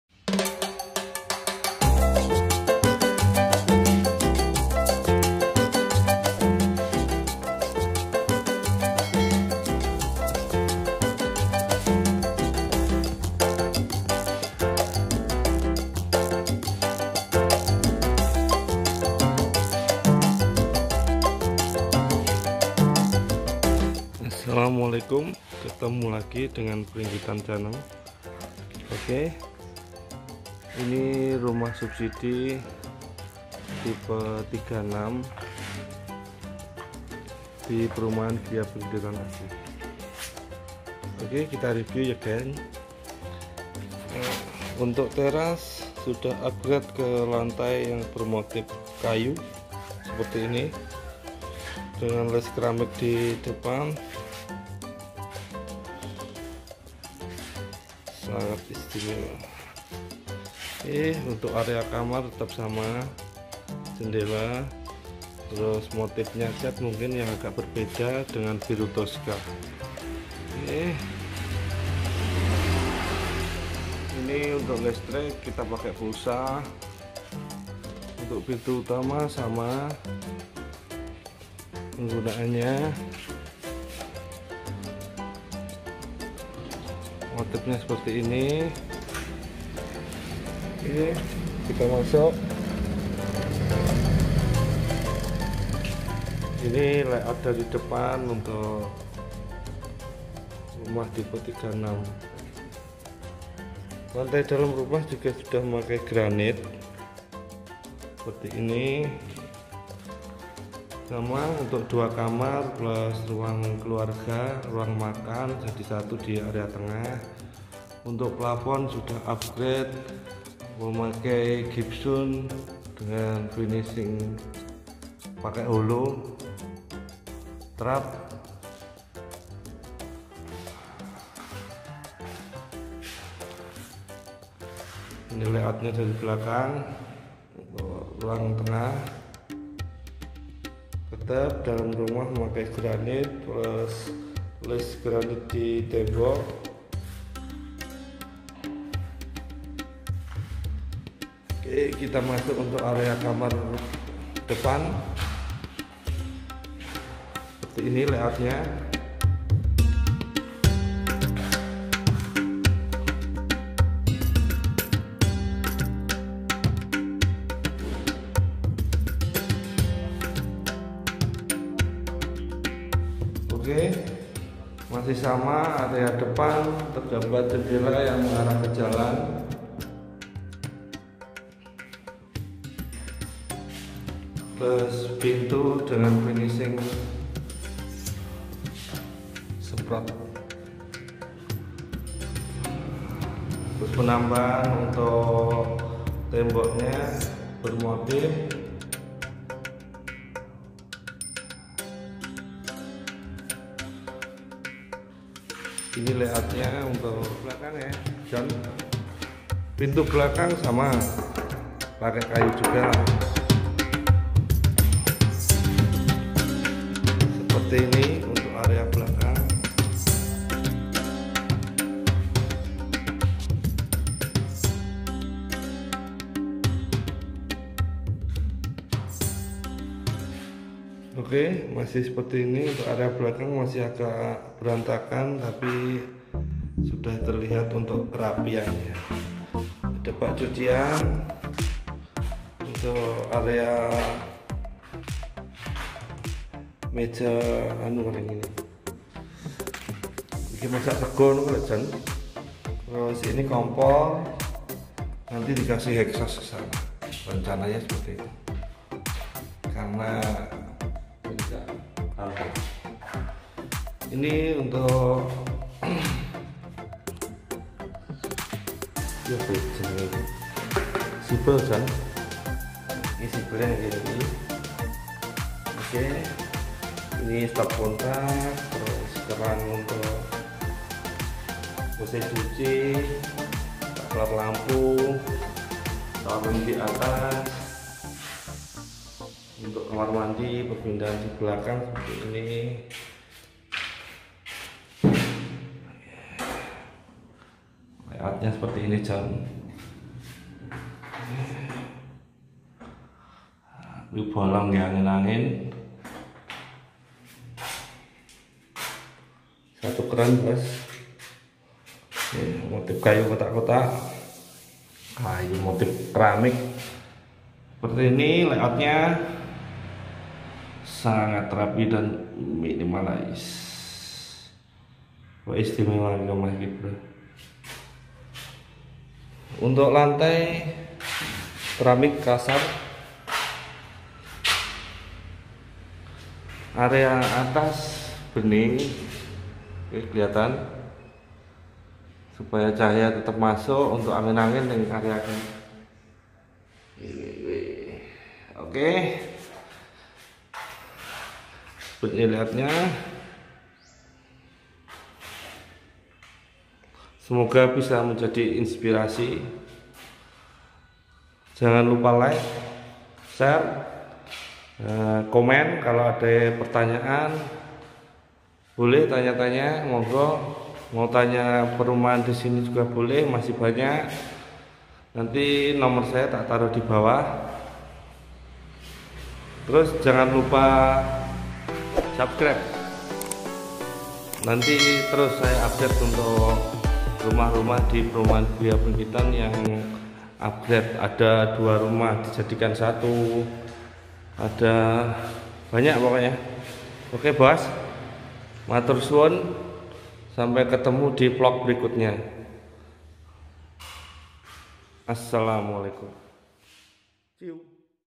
Standard rumah subsidi pakai lantai granit + plafon trap!!!
Pembelian rumah subsidi dengan penambahan lantai granit dan plafon trap terlihat rumah tipe 36 dengan harga non subsidi...!!!
Monggo yg mau kepo untuk rumah tipe 36 yg harganya 165jt lgusung ke nomer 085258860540 anggi
Rumah mewah rumah griya pringgitan asri...